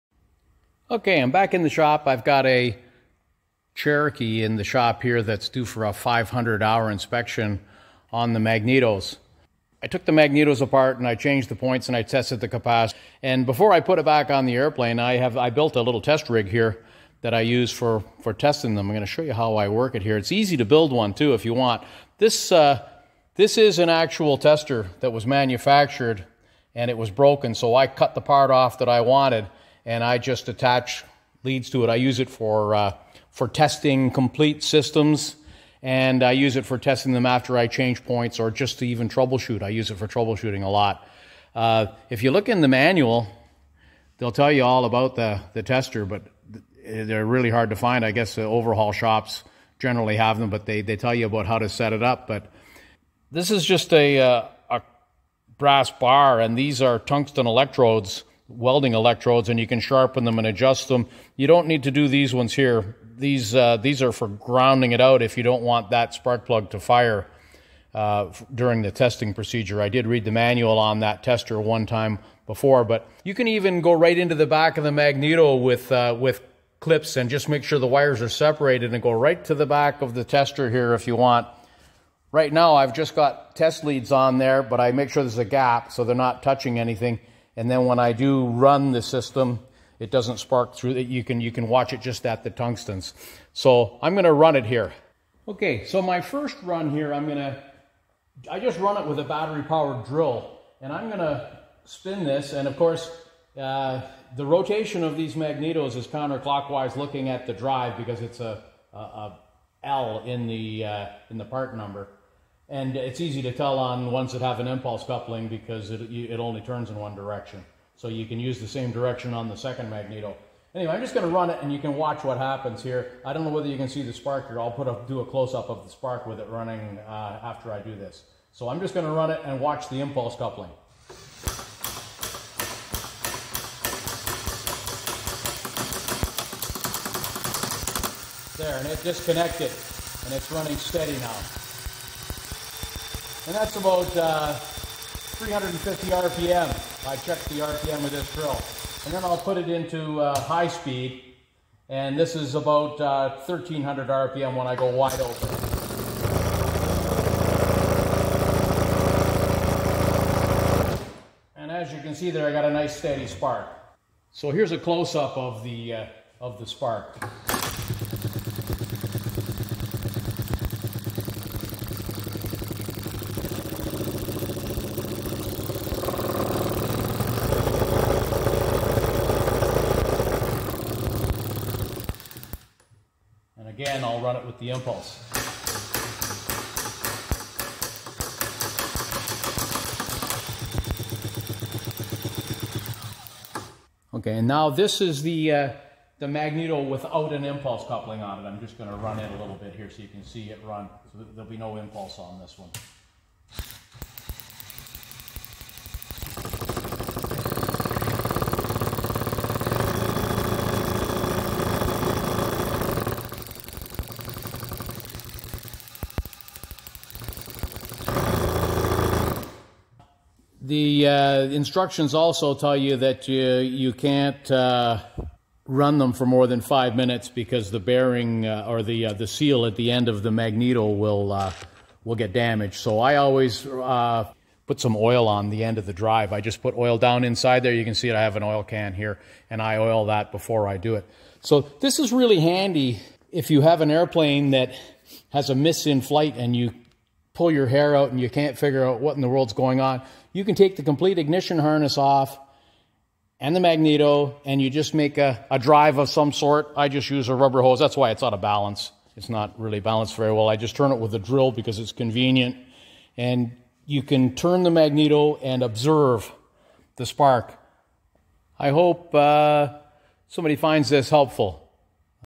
Magneto Testing Aircraft
Slick Bendix TCM Magneto  How I Test Aircraft Magnetos. Please note: I was not careful during the video and allowed the jumpers to cross one another, as a result the the spark was intermittent. I did not think the camera would pick that up.